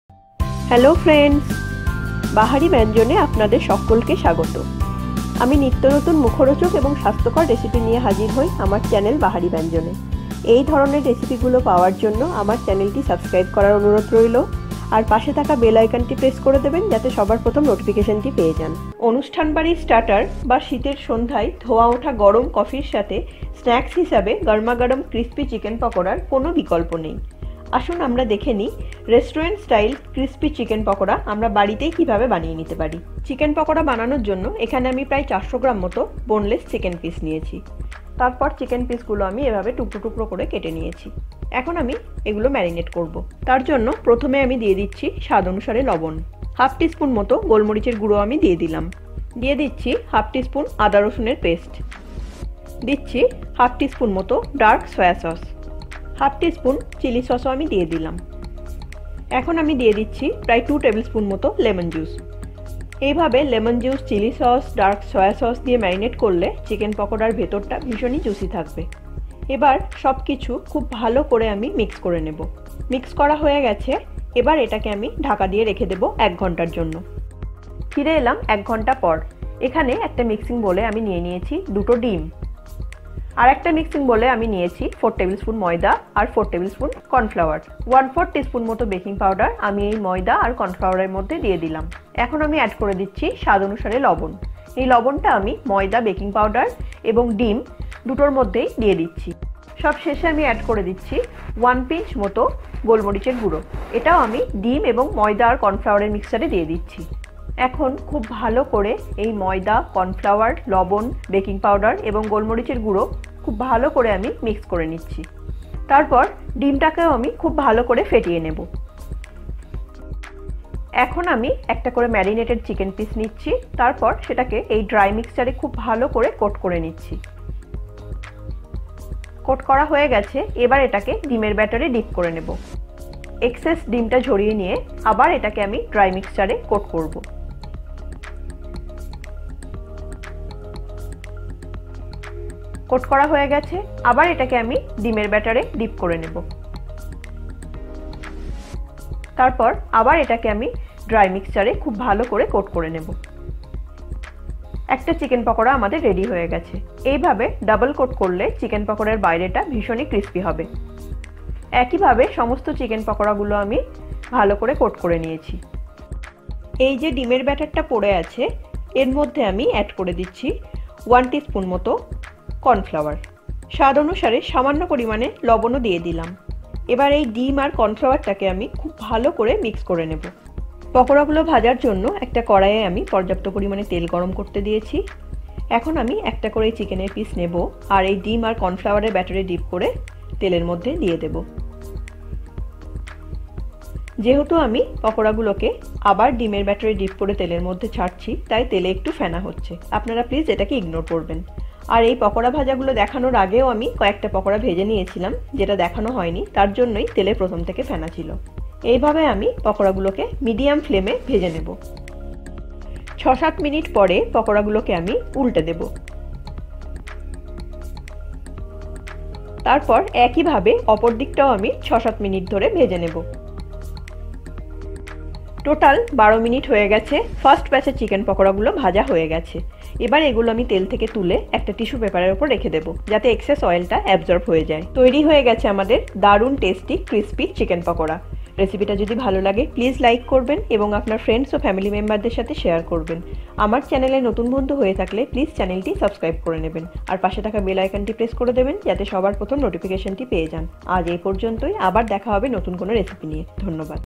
Restaurant Style Chicken Pakora/ বাড়িতেই তৈরি করুন রেস্টুরেন্ট এর মতো চিকেন পকোড়া/ BahariByanjon
Ingredients:
1. Boneless chicken – 400 gm
2. Salt – as per taste
3. Black pepper powder – 3 / 4 th tea spoon
4. Ginger garlic paste – 1 / 2 tea spoon
5. Dark soya sauce – 1 / 2 tea spoon
6. Chilli sauce – 1 / 2 tea spoon
7. Lemon juice – 2 table spoon
8. Cornflour – 2 table spoon + 1 tea spoon
9. All purpose flour – 2 table spoon
10. Baking powder – 1 / 4 th tea spoon
11. White oil – required to deep fry the pakoras